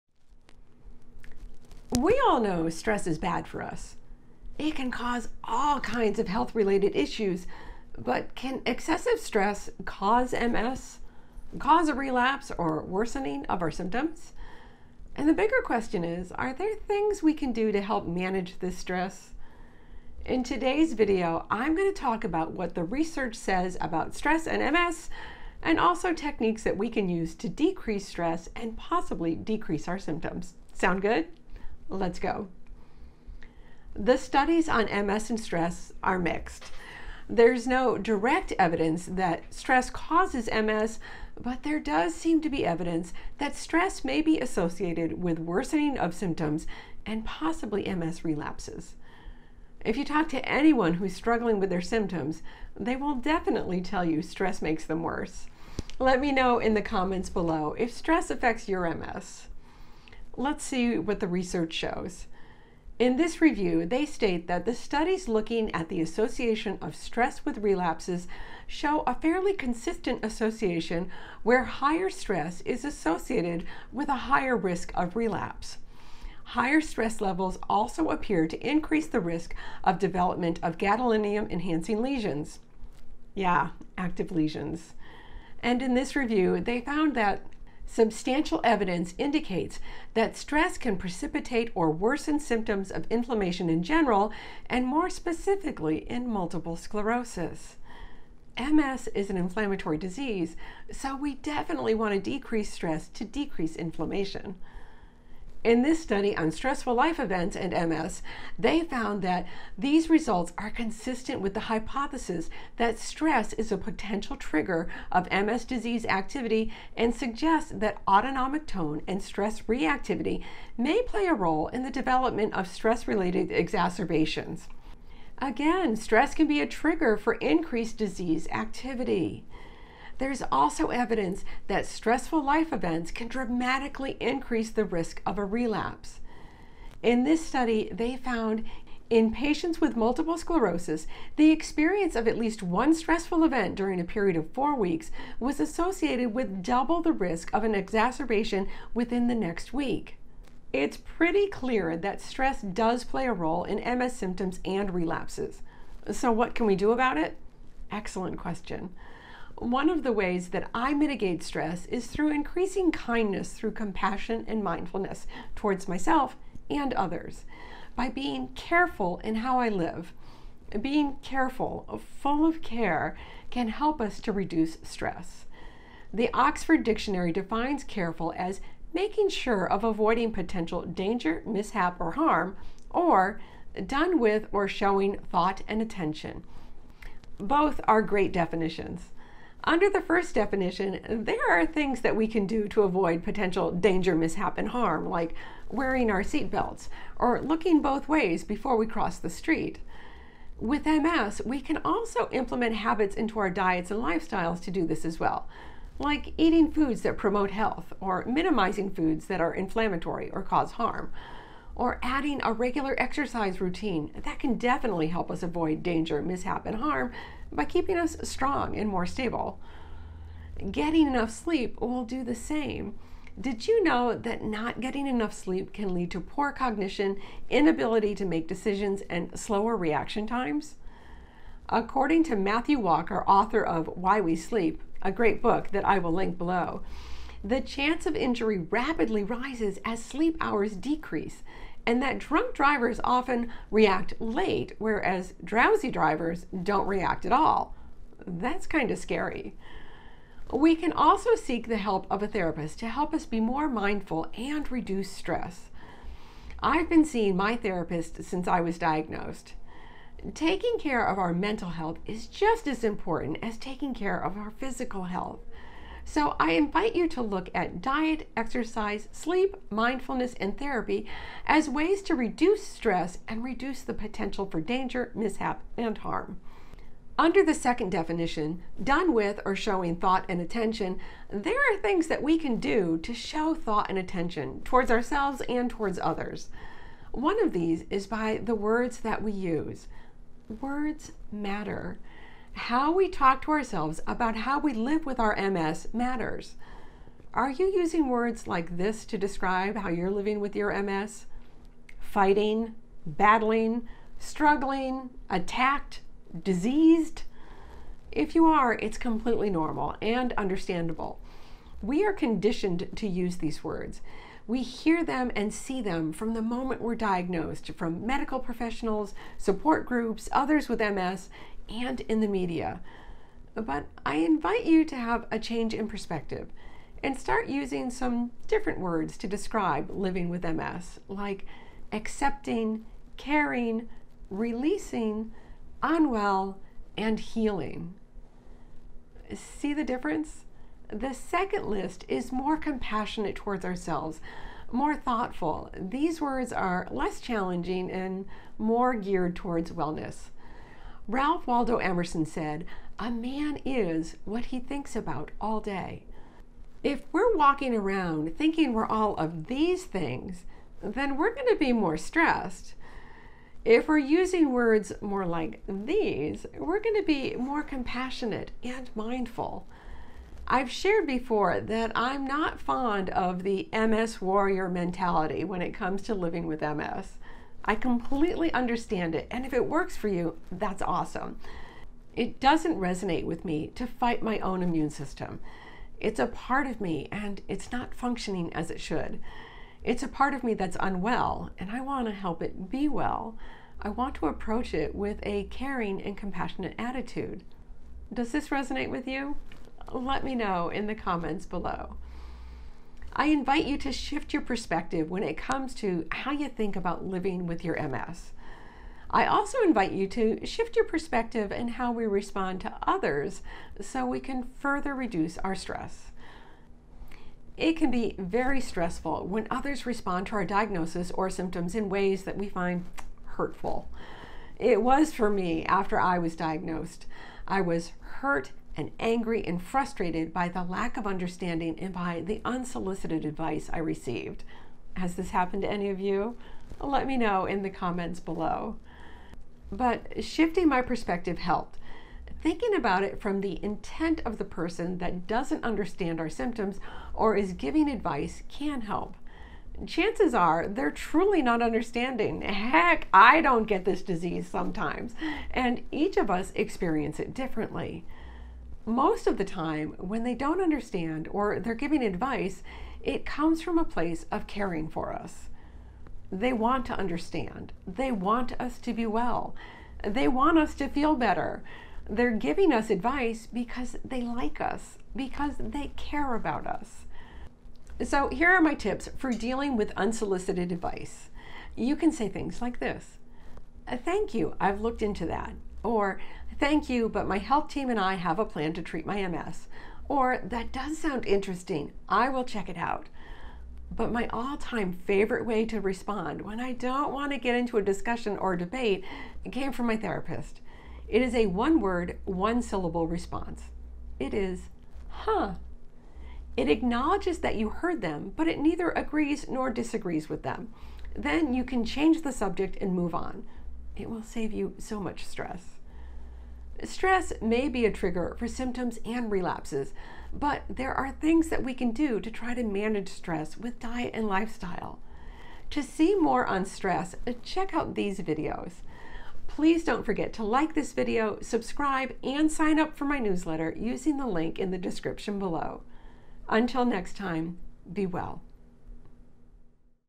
Can Stress Cause an MS Relapse?
Managing stress when we live with multiple sclerosis (MS) can help us to manage our symptoms and may help us reduce the risk of relapses. In this video I share what the research says about stress and how it can affect our symptoms, how it is linked to relapse rates, and how we can help manage stress.